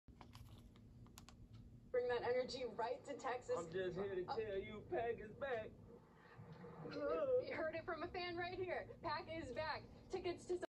Jaire Alexander Just Crashed a Live TV Segment 😂🤣
Pablo (Maher Abucheri) is back on TWP!